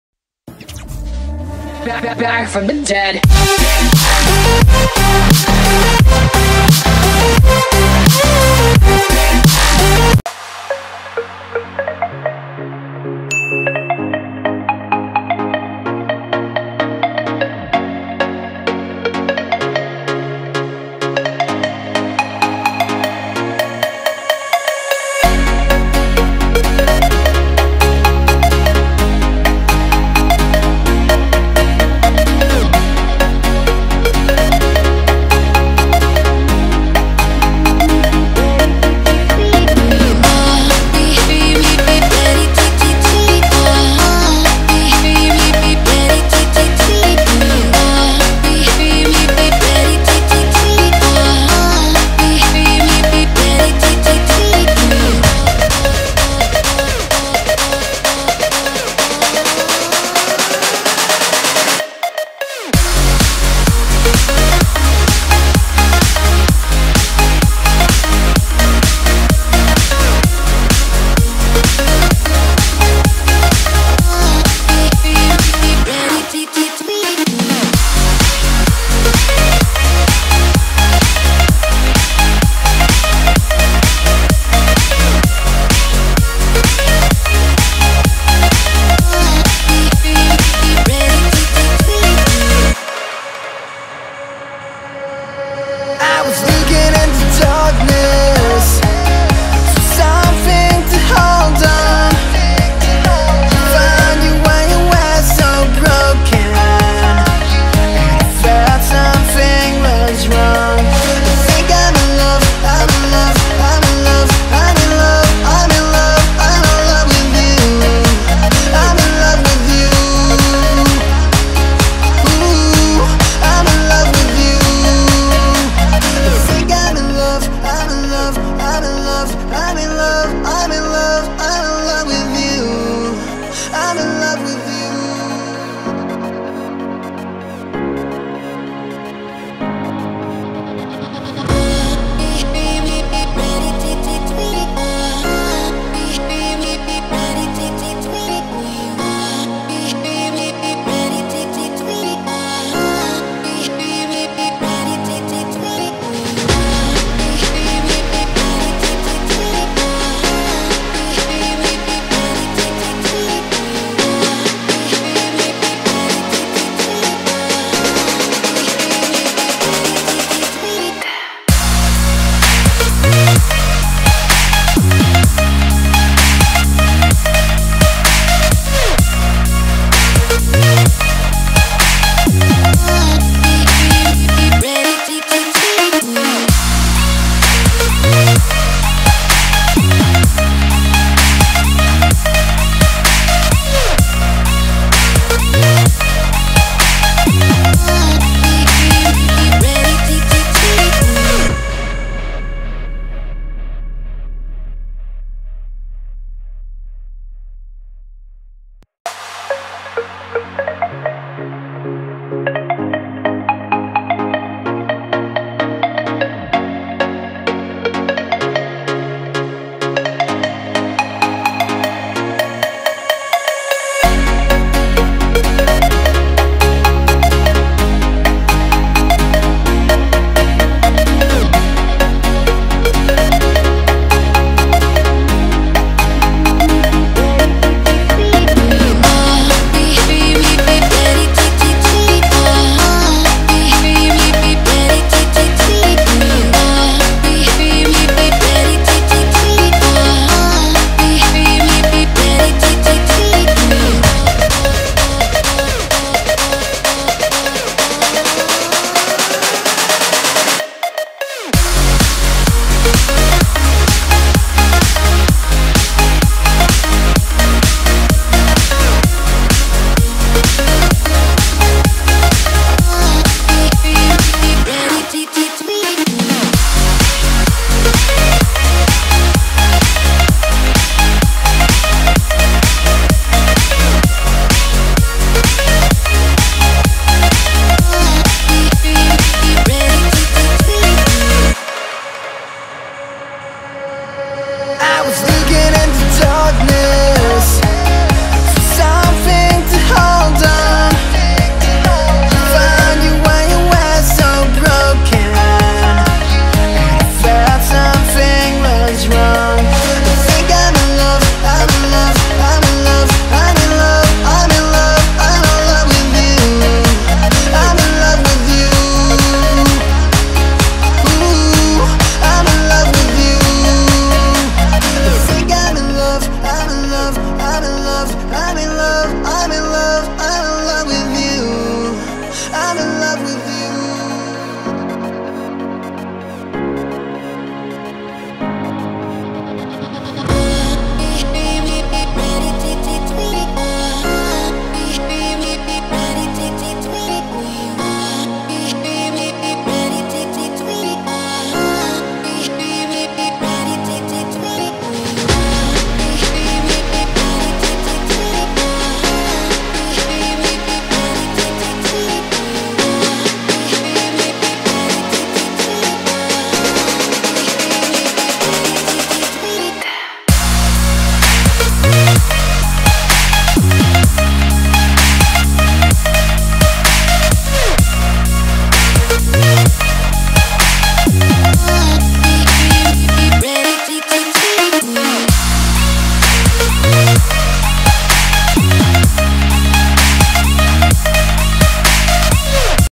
500 SUBCRIBERS SPECIAL !!! Best PVP Texture pack (Deluxe_Bob's texture pack)
Hey guys,Bobdsmc here!
OMG U GUYS ARE THE BEST
This is my new texture pack made from Bobdsmc(me) and Karl